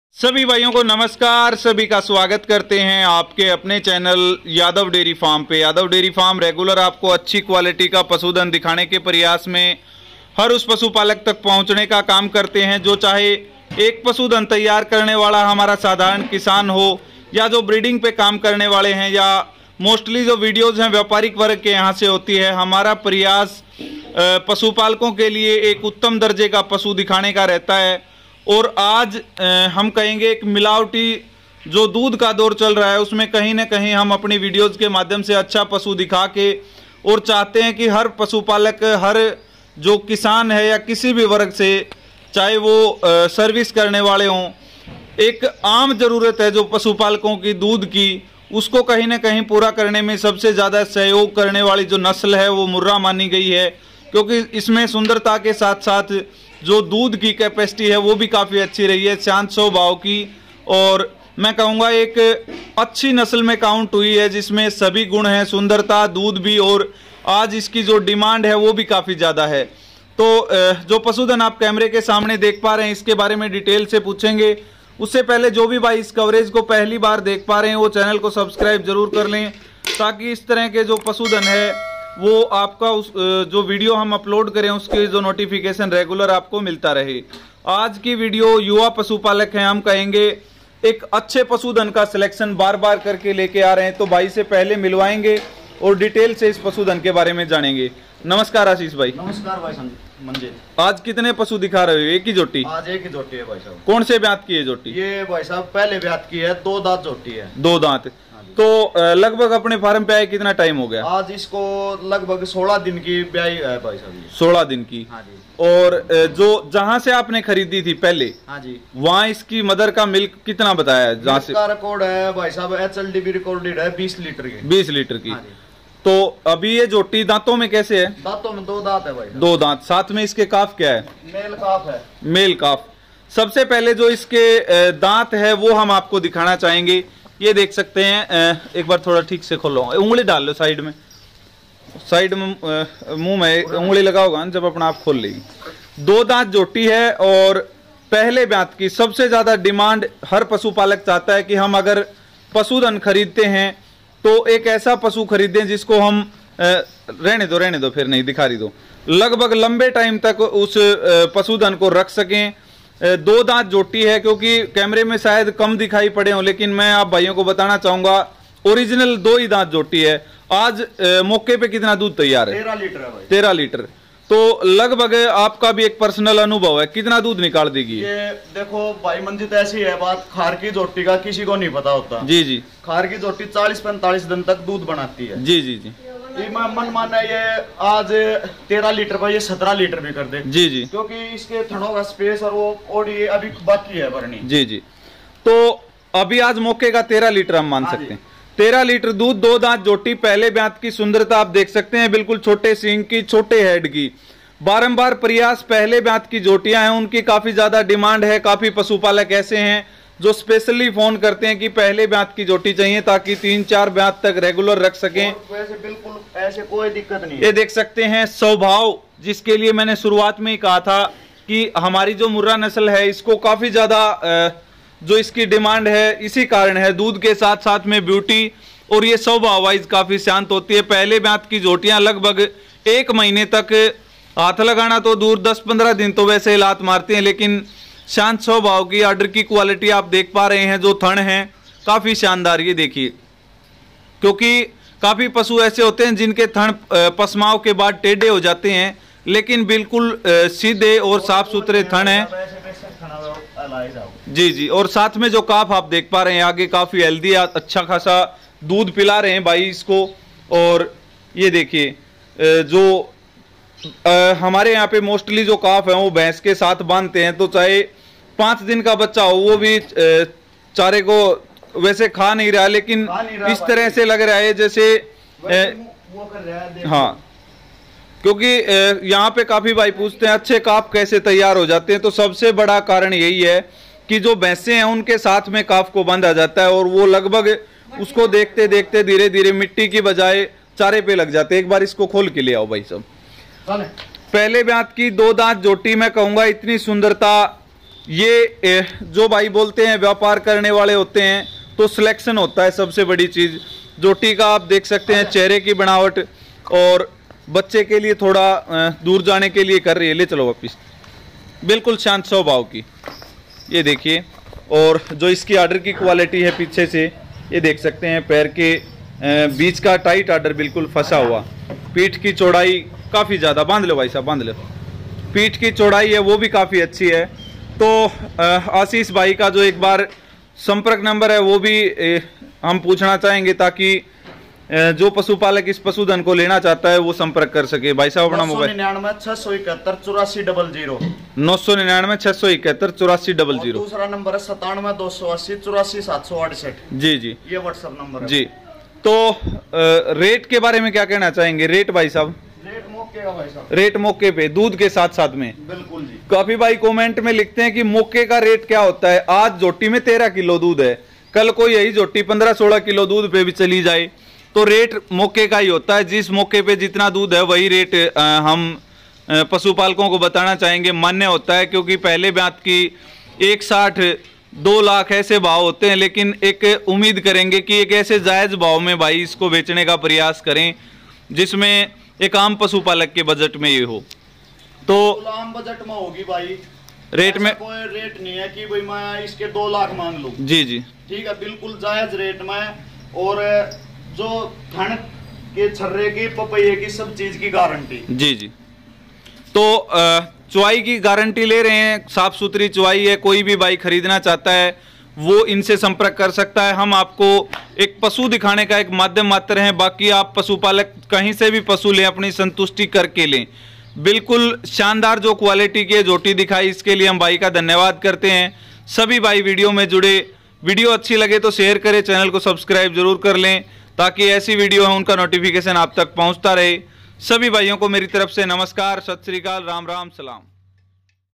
यहां से खरीदे, जबरदस्त भाव में Milk with beauty की (2 teeth) Murrah झोटी (99967-18400) Ashish Sir's
Owner Details.
Ashwani aur Ashish दोनों भाइयों की शानदार झोंकी बिक्री के लिए उपलब्ध

Village.Baghpur
District.Jhajjar
Haryana (india)